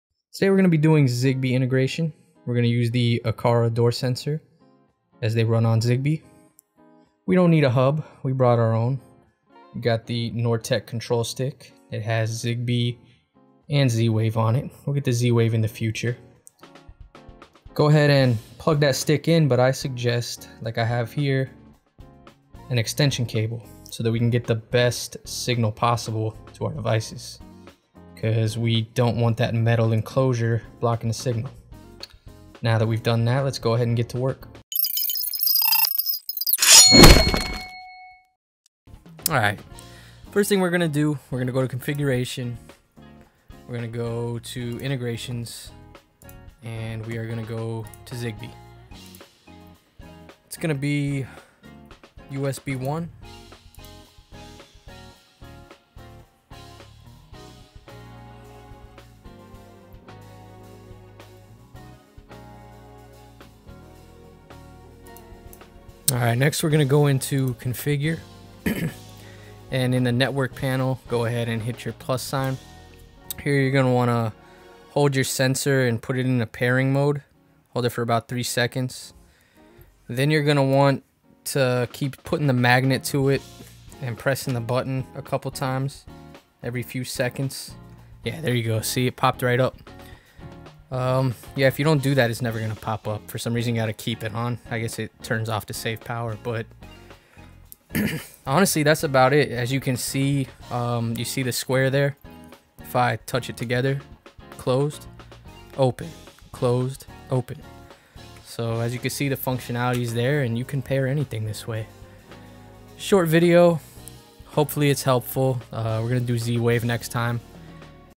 Home Assistant Zigbee Integration with HUSBZB-1 featuring Aqara Sensor
In today's video we will add the ever popular zigbee functionality to home assistant.  Using the HUSBZB-1 stick we no longer require a hub to use our zigbee and z-wave devices, reducing clutter and removing more of our smart home from the cloud.  We will be using the zigbee based Aqara door sensor to display functionality.

GoControl HUSBZB-1
Xiaomi Aqara Door/Window Sensor